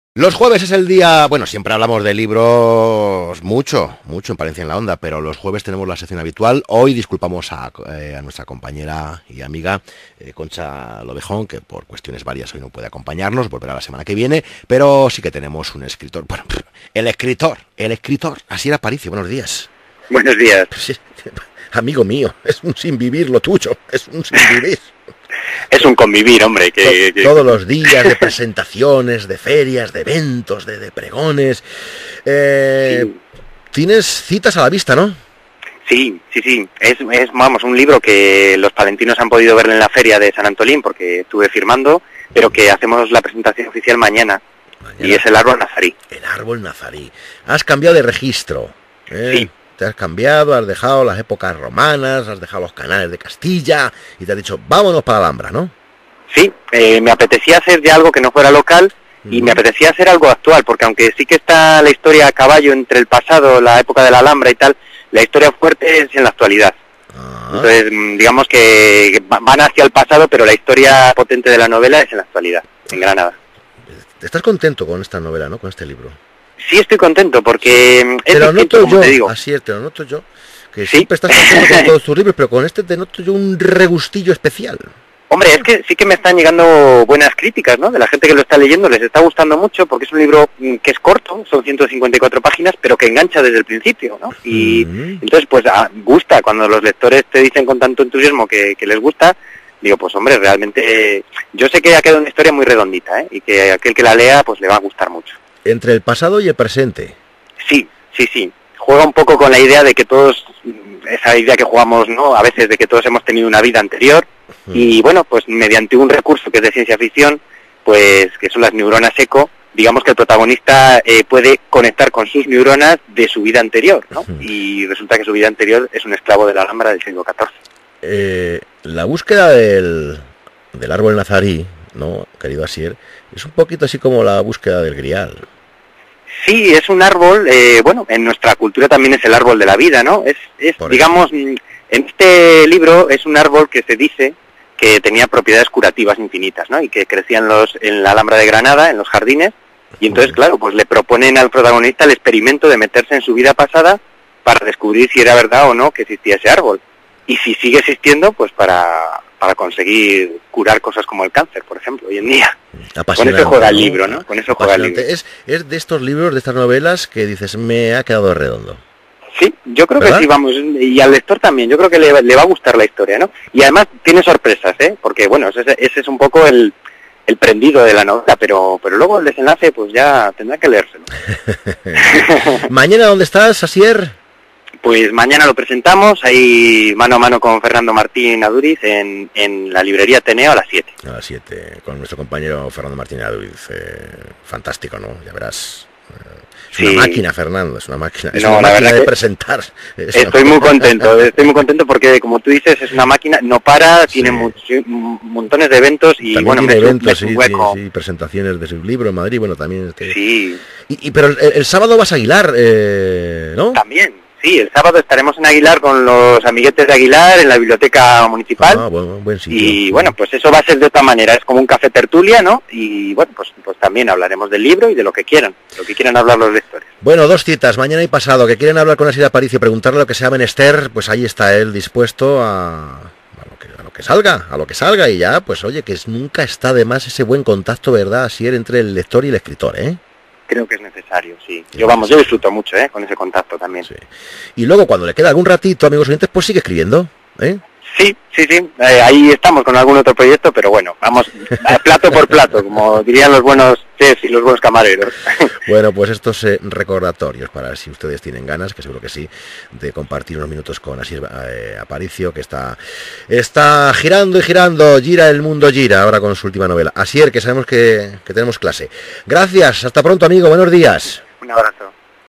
Entrevista OndaCero El árbol nazarí
Entrevista de Julio Izquierdo en Onda Cero Palencia al escritor Asier Aparicio con motivo de la presentación de su novela "El árbol nazarí".